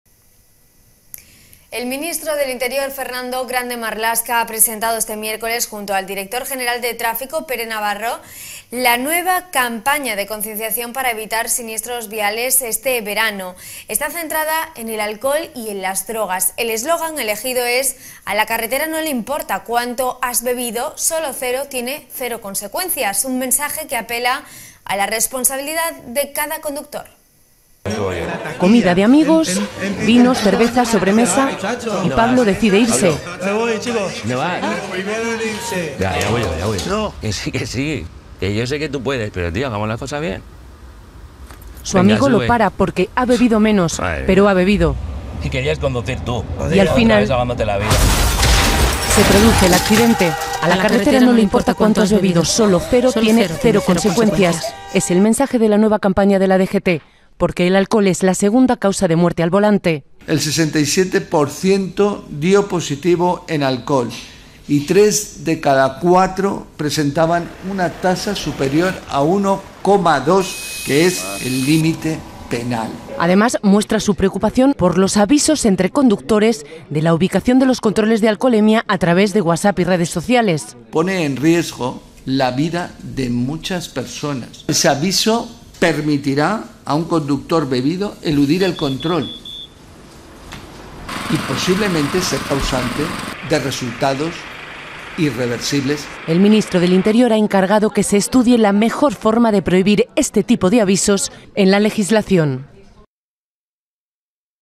🔴Noticia - La DGT lanza su campaña de verano centrada en el alcohol y las drogas
El ministro del Interior, Fernando Grande-Marlaska, ha presentado este miércoles junto al director general de Tráfico, Pere Navarro, la nueva campaña de concienciación para evitar siniestros viales este verano, centrada en el alcohol y las drogas. El eslogan elegido es 'A la carretera no le importa cuánto has bebido. Sólo cero tiene cero consecuencias', un mensaje que apela a la responsabilidad de cada conductor.

English:

The Minister of the Interior, Fernando Grande-Marlaska, presented this Wednesday together with the General Director of Traffic, Pere Navarro, the new awareness campaign to avoid road accidents this summer, focused on alcohol and drugs. The chosen slogan is 'The road doesn't care how much you've drunk. Only zero has zero consequences', a message that appeals to the responsibility of each driver.

Live streaming